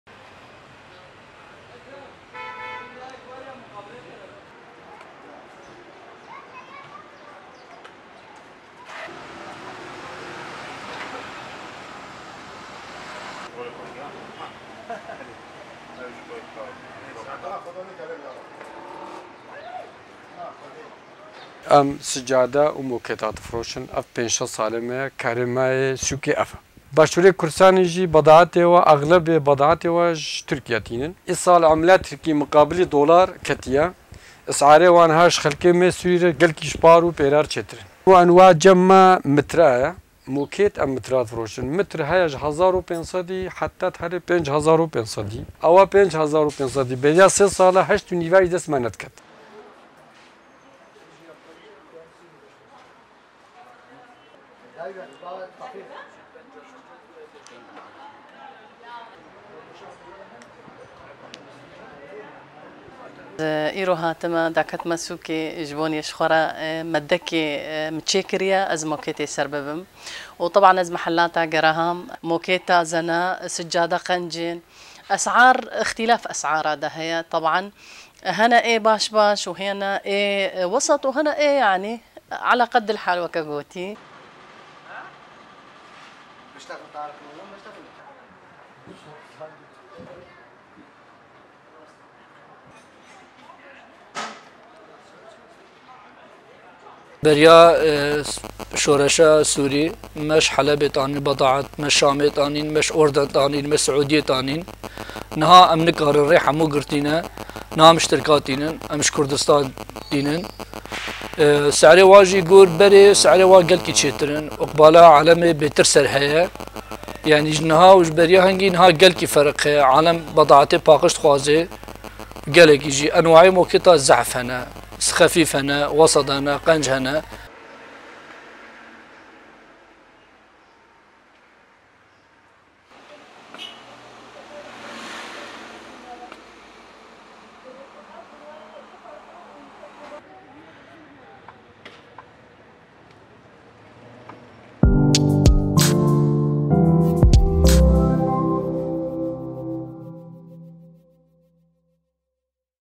الإقبال على شراء السجاد في ديريك
آسو-آريا حاجي

ازداد إقبال أهالي مدينة ديريك / المالكية على شراء السجاد في السنوات الأخيرة.
ما هي أسعار السجاد؟ ومن أين مصدر هذه البضاعة؟
المزيد في التقرير التالي: